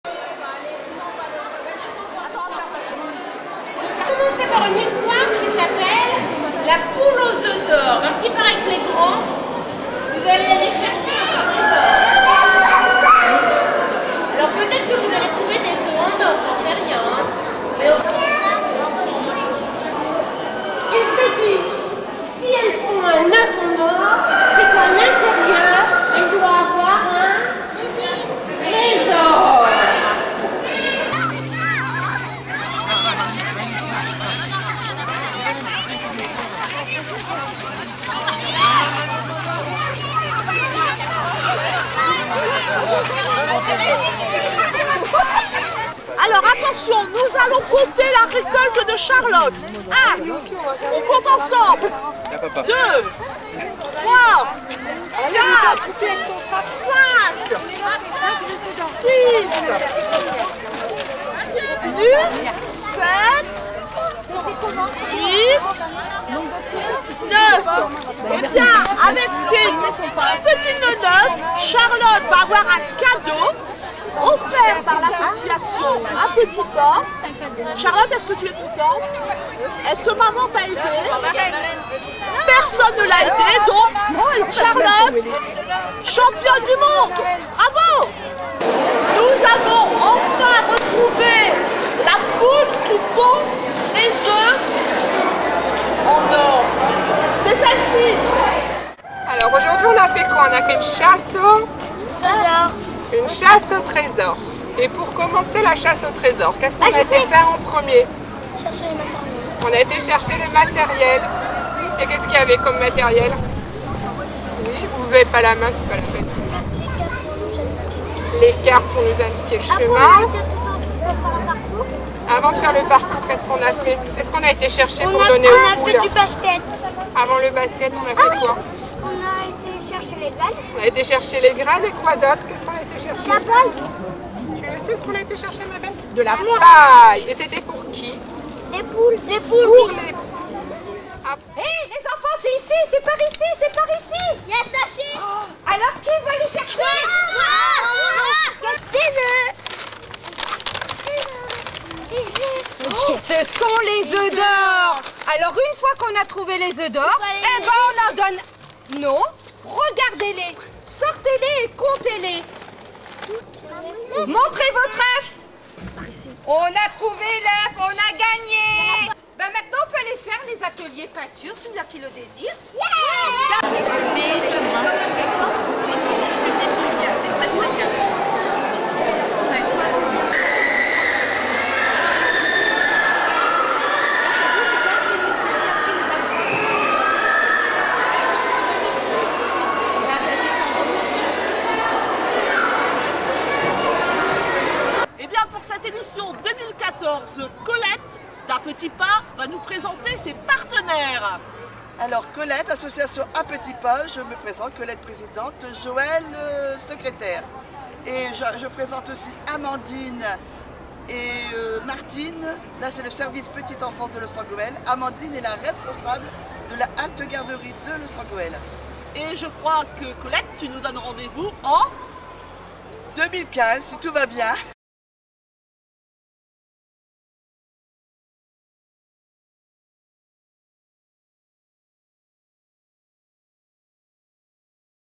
LOOS TV - La chasse aux œufs
Partez à la quêtes des œufs puis arretez vous quelques instants pour un atelier peinture.